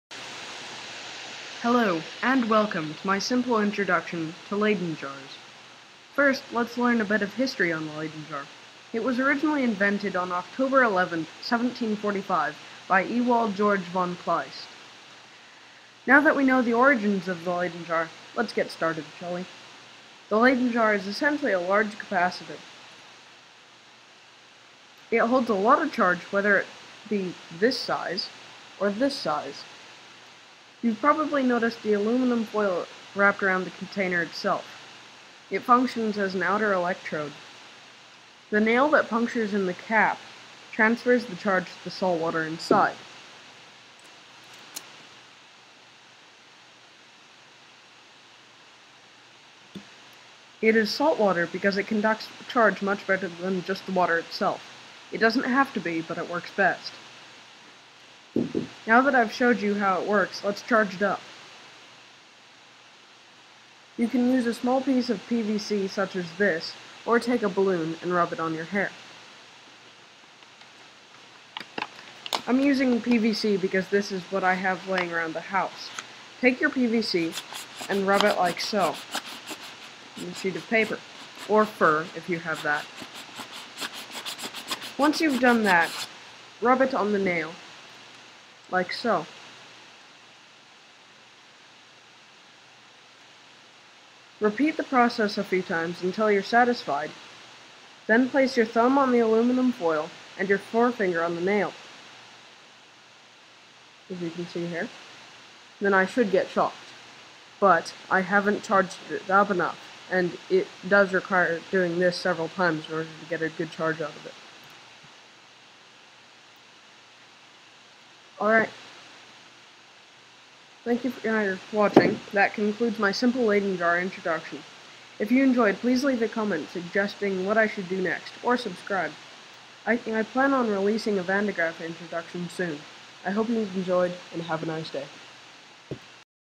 Simple Leyden Jar Introduction
In this video, I demonstrate how a Leyden Jar works as well as give some insight on the history of this fantastic invention.